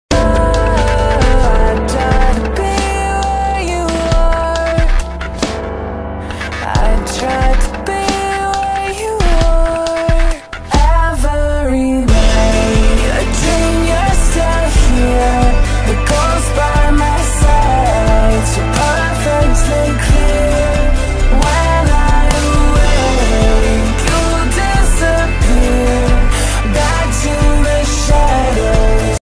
Loki ✘ Belle: I'd d i e to be where you are;;
This is my latest part for Haunted Heroine Studios I loved vidding this theme, though I had a bit of trouble for a while, inspiration-wise. Basically, the heroine loses the villain, and she grieves his death so strongly that she still sees him in her dreams. I chose Loki x Belle since I thought that'd be easiest to get that theme across (though I did almost pick Elsa and Loki. The only reason I chose Belle was because she's more emotional/I'm more familiar with her scenes). Anyway, I hope you enjoyed!